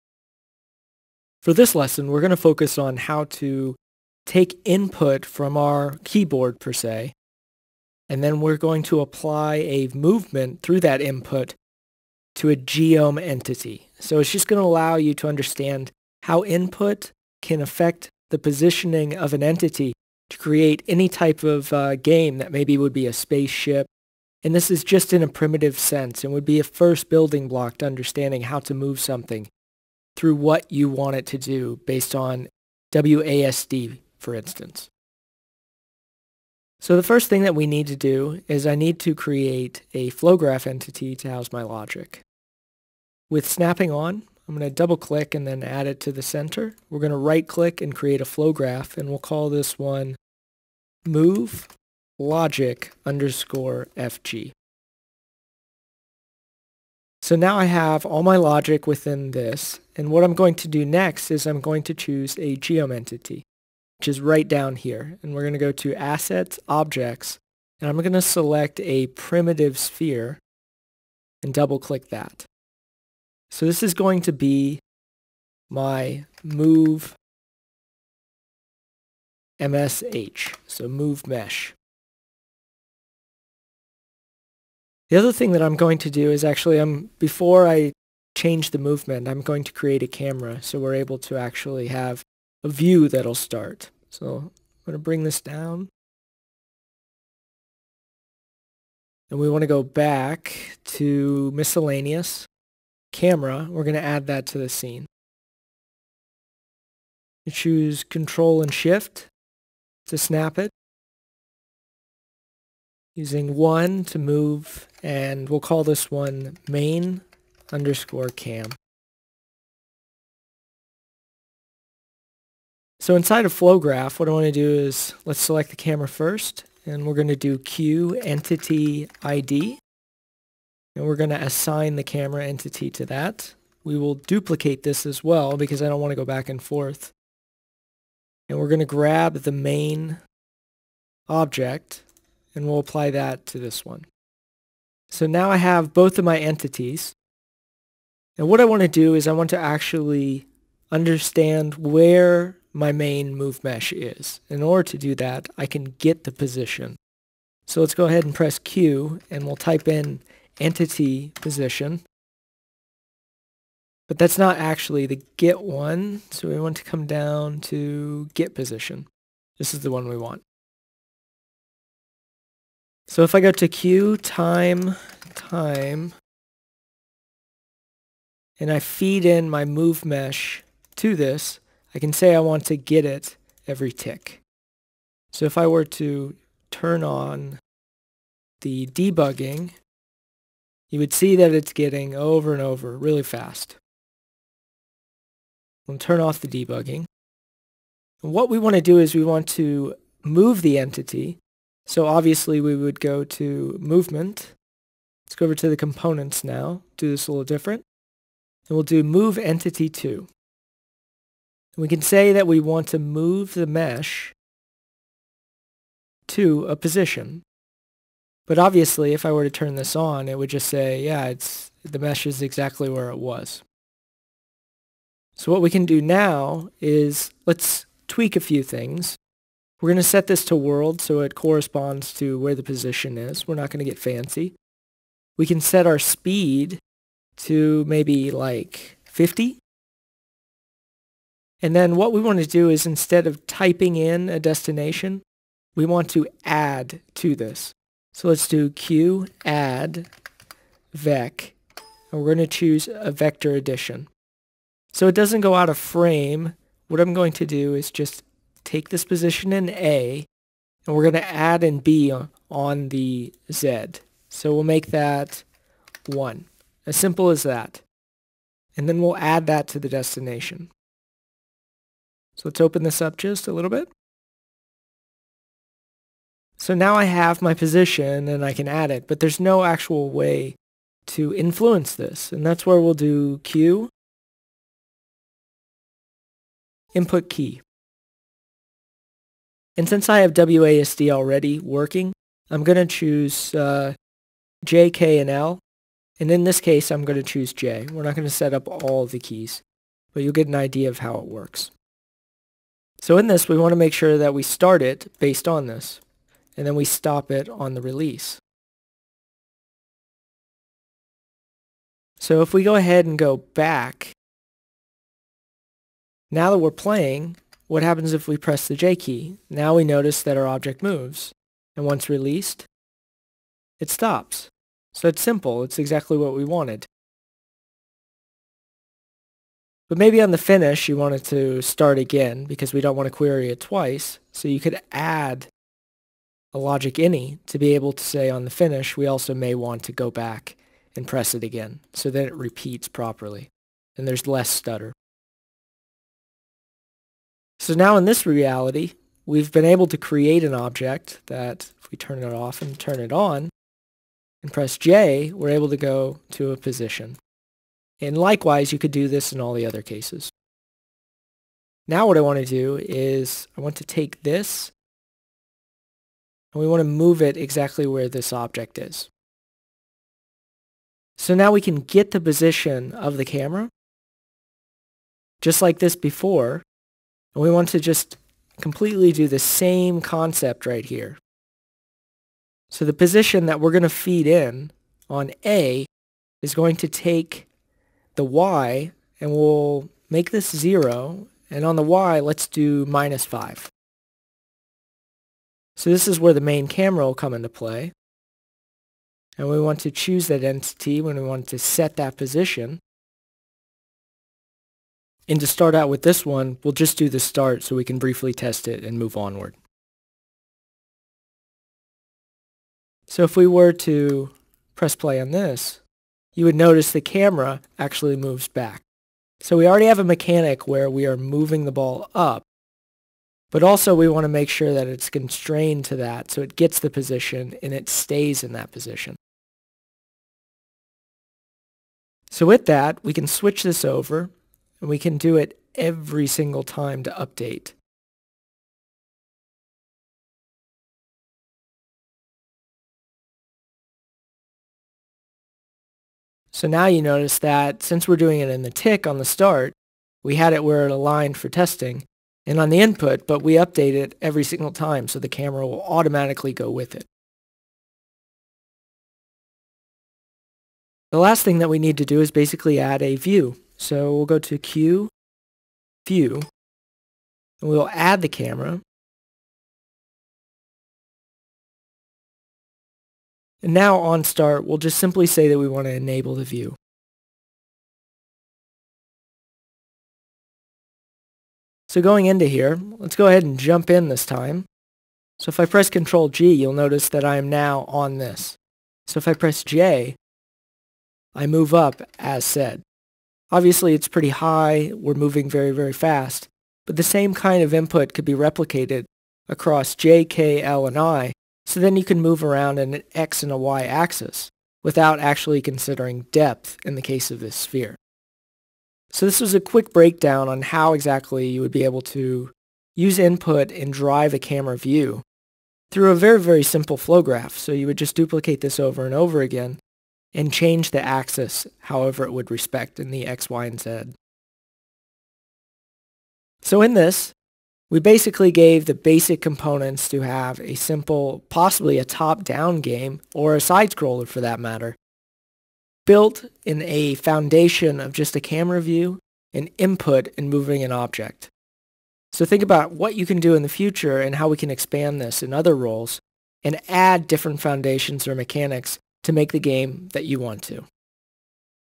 How to bind inputs to actions in CRYENGINE | Game & Level Design
In this lesson we cover how to use input nodes to drive the vector movement of a geom entity. We then explore how to extend this same practice to a camera and then force the view on game start. With these basic concepts most games can be reproduced.